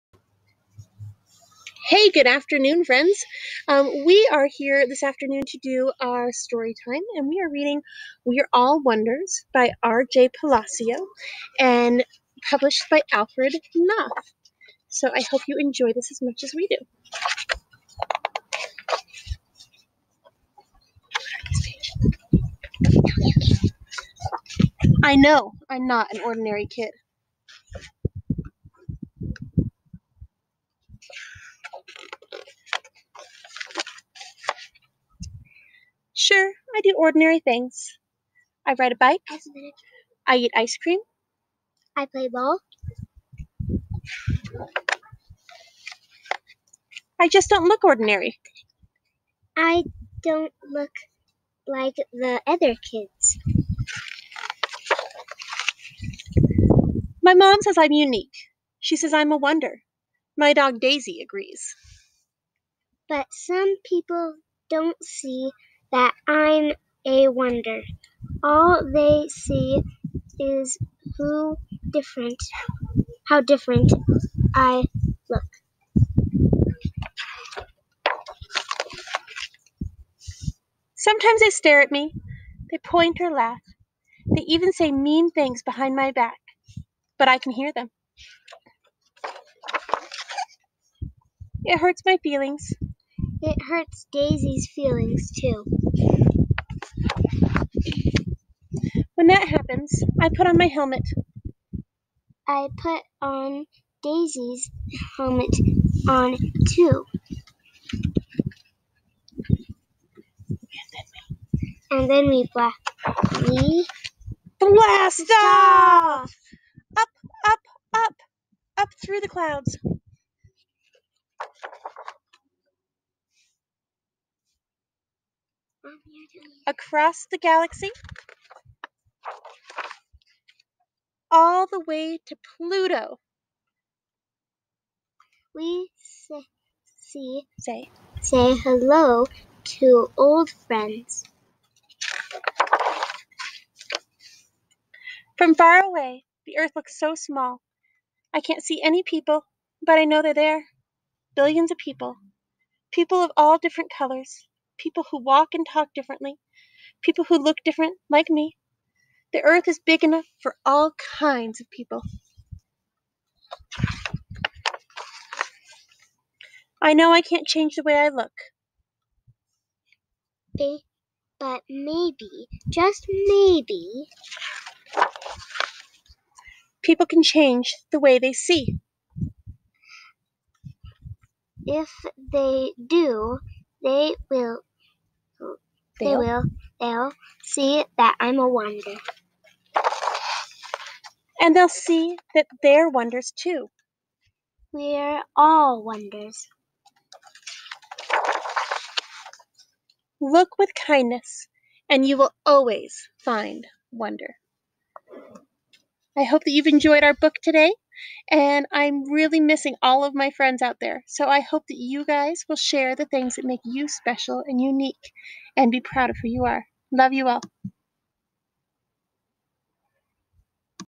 Story Time with Mrs. Waage - We're All Wonders
We're All Wonders - by R.J. Palacio
Published by Alfred A. Knopf